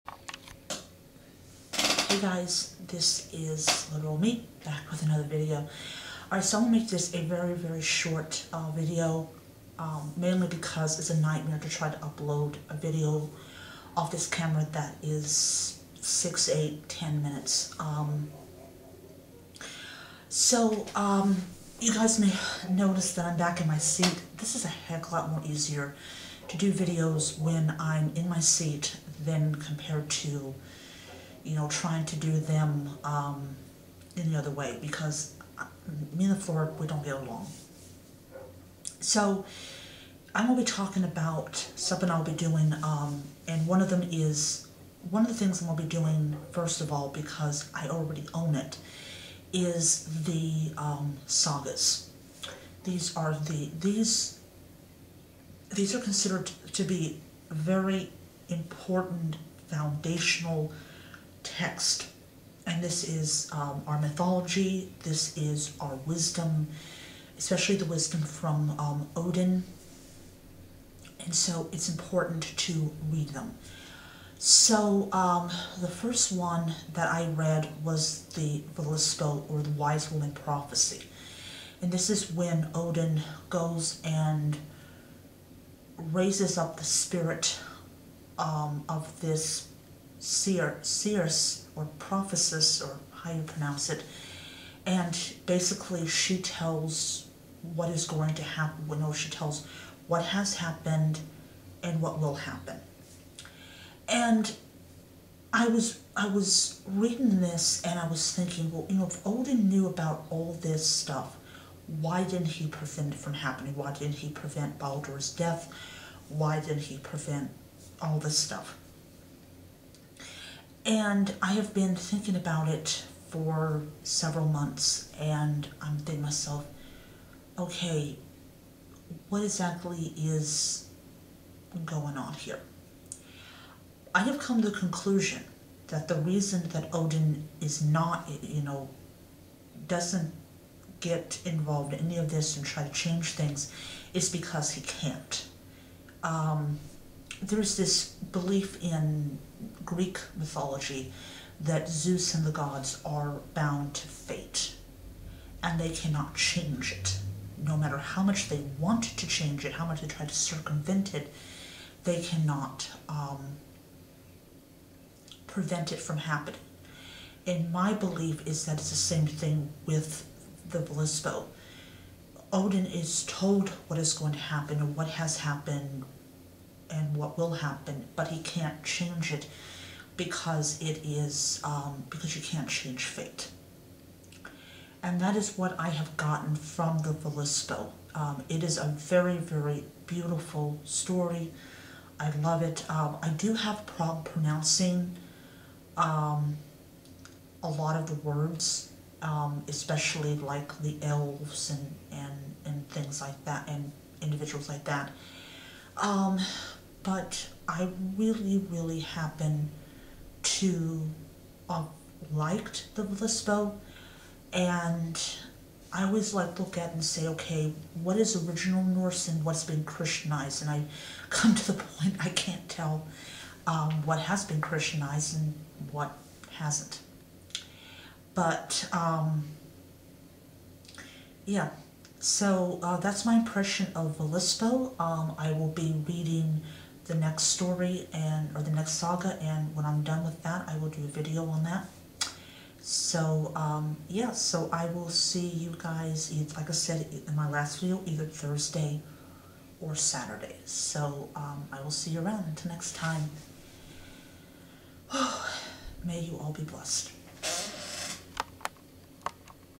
Talking about Voluspo
I talk about the first book in the Poetic Edda.